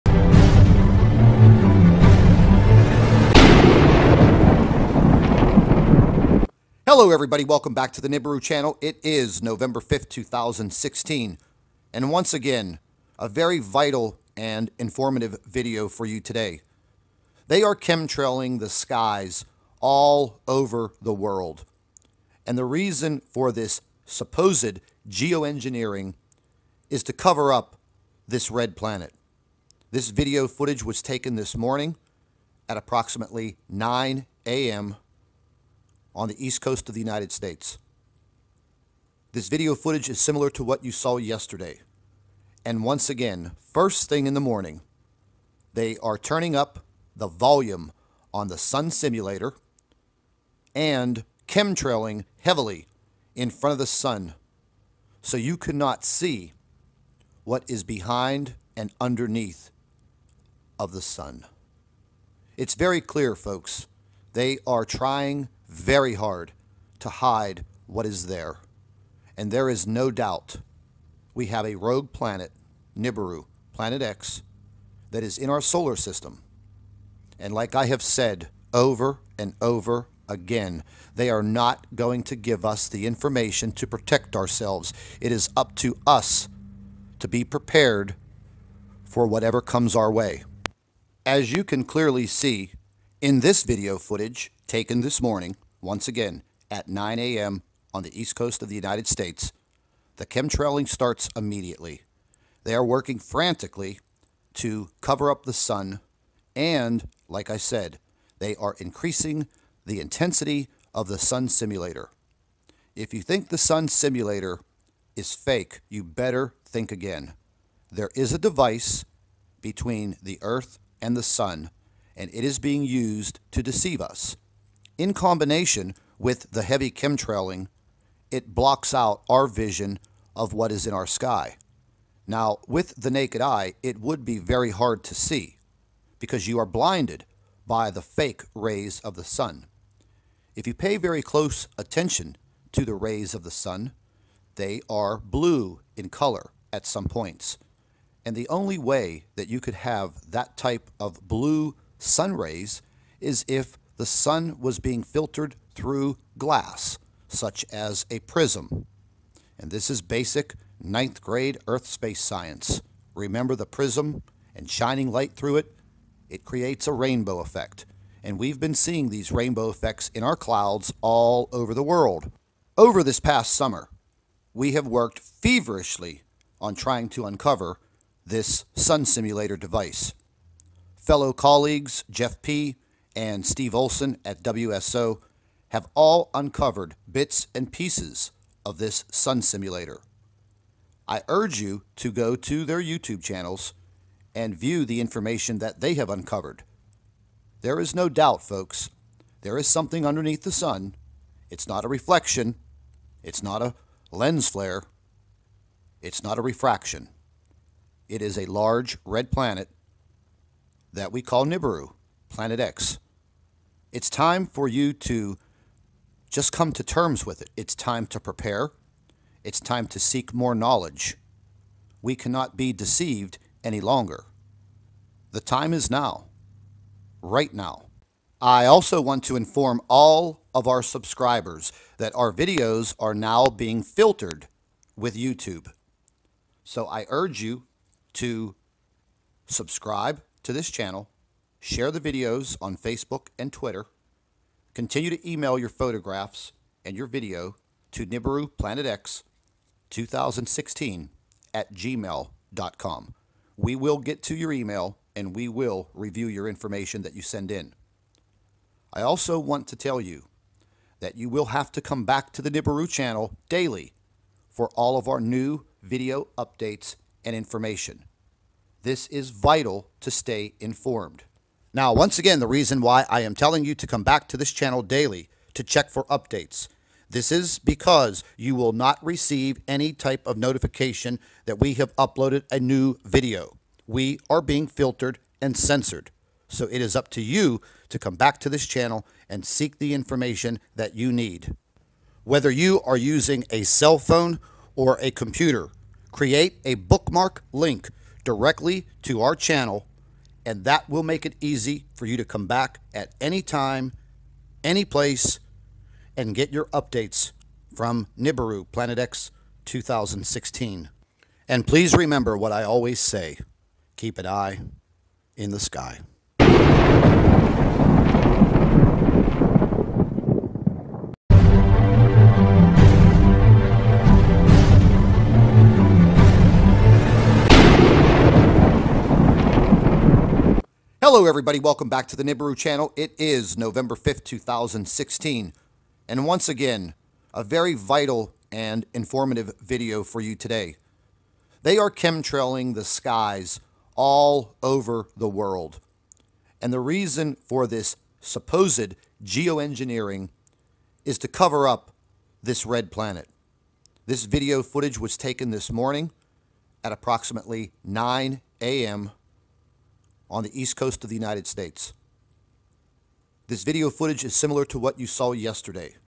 PLANET X CHEMTRAILING TO COVER UP THE RED PLANET NIBIRU!
This form of daily chemtrailing is not part of any geoengineering it is simply to cover up Nibiru Planet X in our skies... This is part of the daily deceit that we go through on planet Earth as human beings... Once again we are being lied to and this is the biggest cover-up in human history.